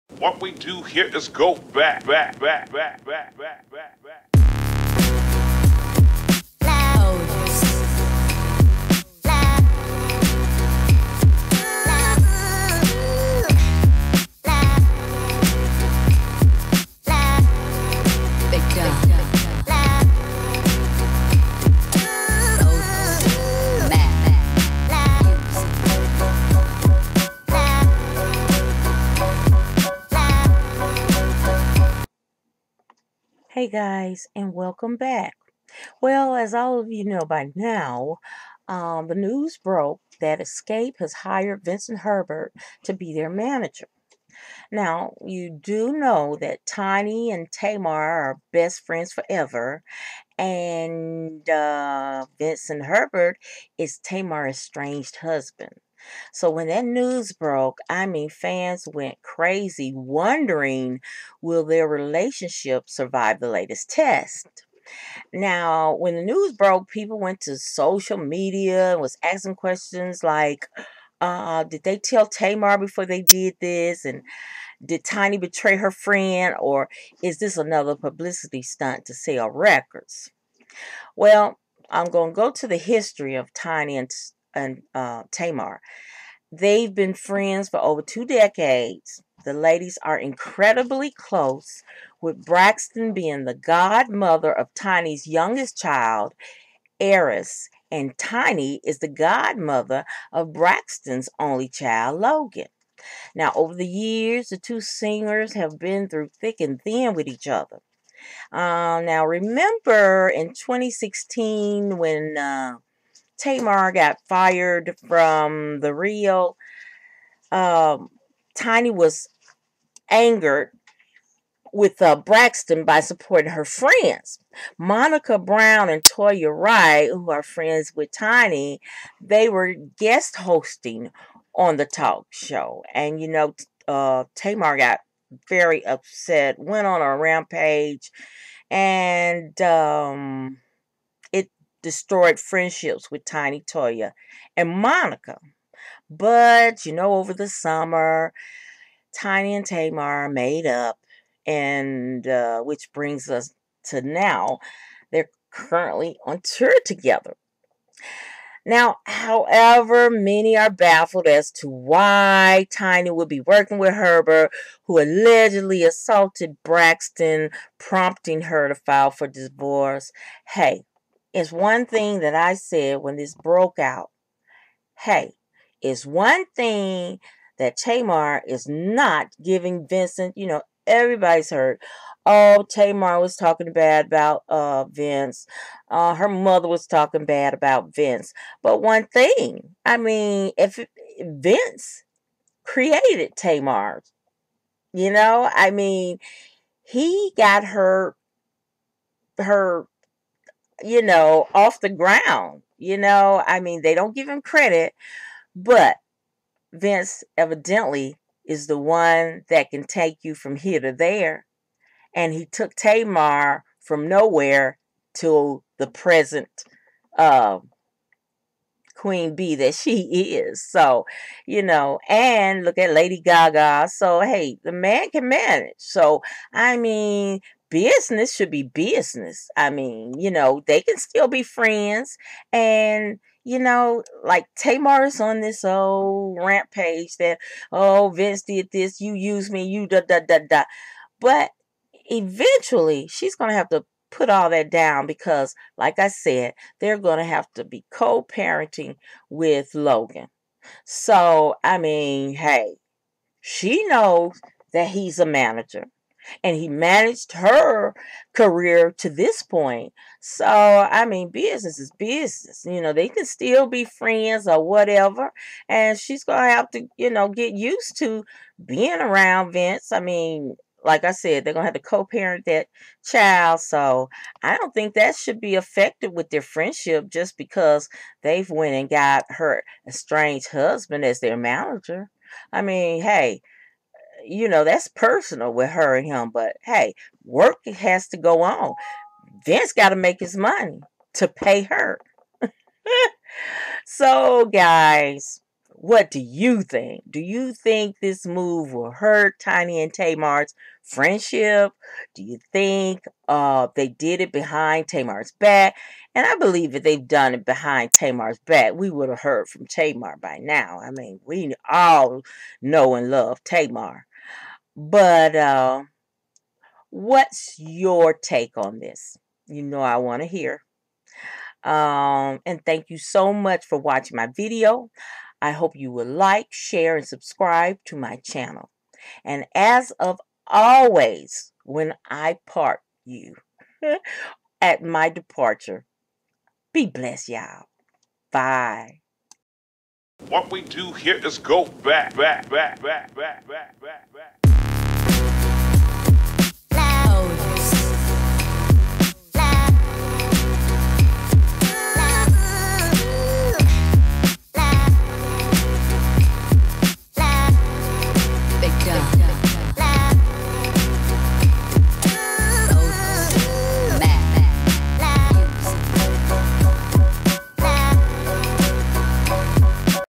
TAMAR BRAXTON & TINY HARRIS FRIENDSHIP IS BROKEN NOW THAT VINCE HERBERT IS XSCAPE MANAGER
WILL TAMAR & TINY STILL BE BEST FRIENDS SINCE VINCE IS XSCAPES'S MANAGER